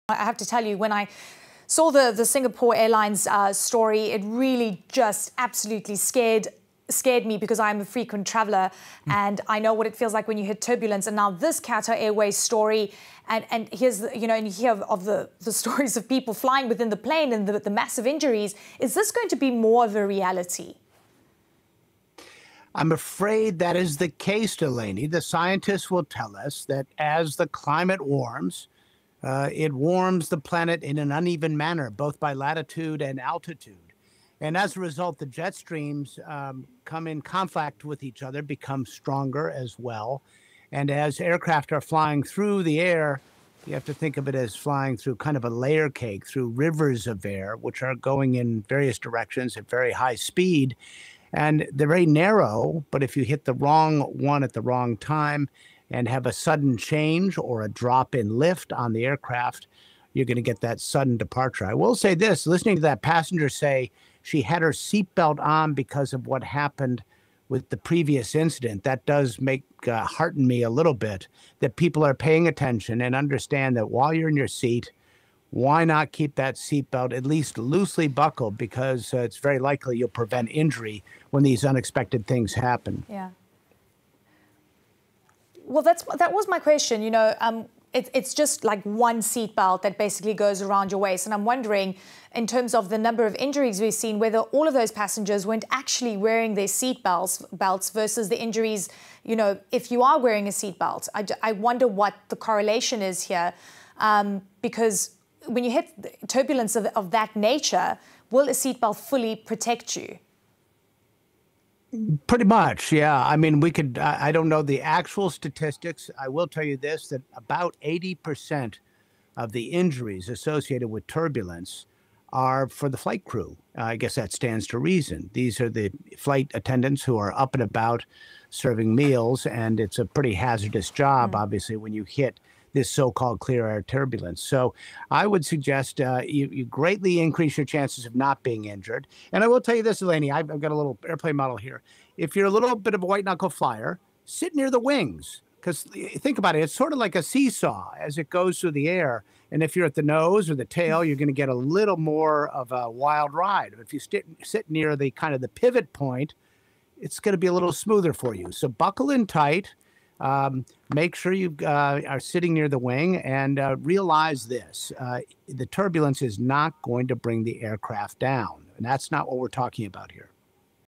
هل يُمكن للمطبات الهوائية الشديدة أن تُسقط طائرة؟ خبير طيران يوضّح
في الآونة الأخيرة، أُصيب عدد من ركّاب رحلة الخطوط الجوية القطرية في رحلة متجهة من العاصمة القطرية الدوحة إلى عاصمة أيرلندا دبلن بعد أن تعرضت الطائرة لمطبات هوائية. ويأتي ذلك بعد أيام من إصابة عدد كبير من الركاب ومقتل رجل كان يعاني من مرض في القلب على متن طائرة تابعة للخطوط الجوية السنغافورية، بعد تعرّضها لمطبات هوائية شديدة أيضًا.
محلل الطيران في CNN، مايلز أوبراين، يشرح كيف يؤدي تغيّر المناخ إلى حدوث المزيد من الاضطرابات الجوية، ويوضّح المكان الأكثر أمانًا للجلوس على متن الطائرة، كما يبيّن ما إذا كان يمكن للمطبات الهوائية أن تتسبّب بسقوط الطائرة.